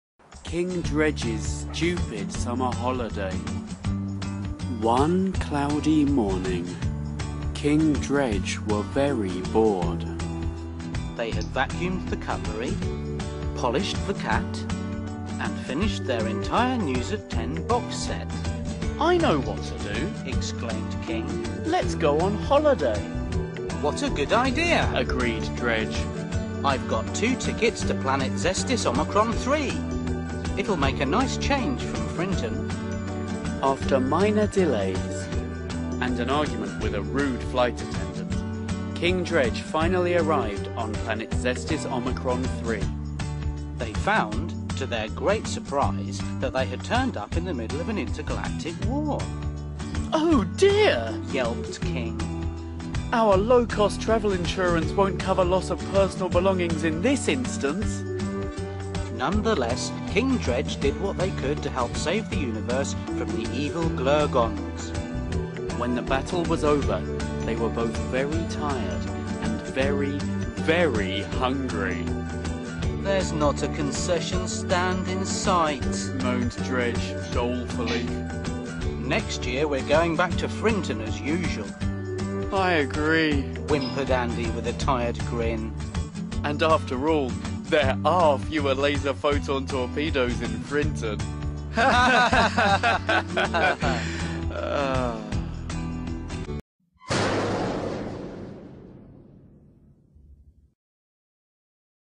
Our Summer Holiday (With Aliens)
King Dredge try somewhere new for their summer holiday this year. But does it involve aliens? If not, the title of the video is miselading.